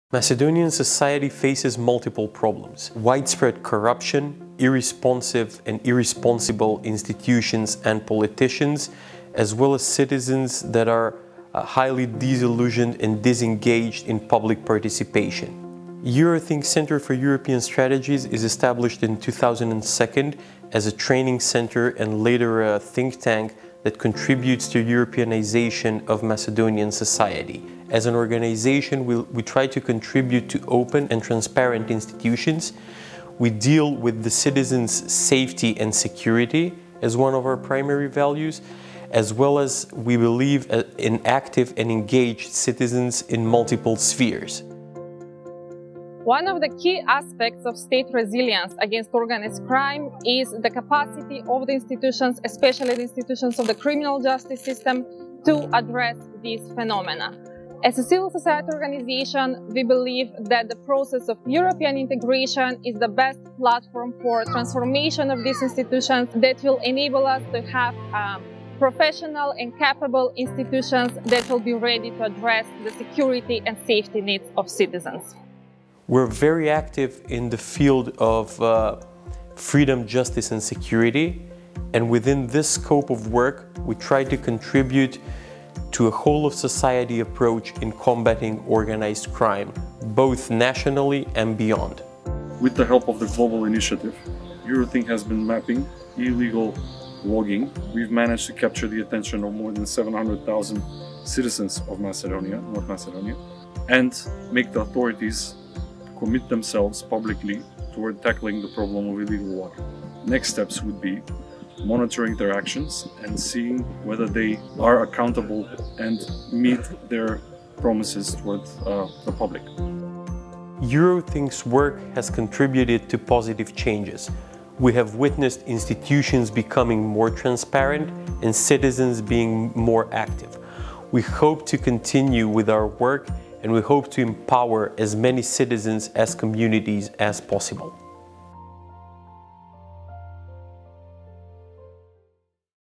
EUROTHINK in North Macedonia: The resilience of civil society organizations in the Western Balkans
EUROTHINK - European Center for European Strategies - have being working to make institutions in North Macedonia more transparent, and have encouraged citizens to become more active and engaged.

With the help of the Global Initiative, they have been mapping illegal logging. EUROTHINK engaged more than 700,000 citizens, who called on the authorities to commit themselves publicly to tackling the issue.

Read more about the work of civil society organizations in the Western Balkans